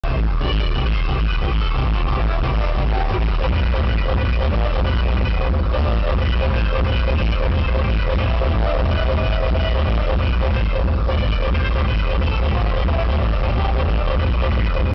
Video Footage from Voodoo Fest 2003 Day 3 in New Orleans Clip 19
Tim Walsh performs on stage at the Playstation 2 Dance Tent in City Park in New Orleans, LA for the Voodoo Music Experience 2003.

Juston Western
Videographer: Juston Western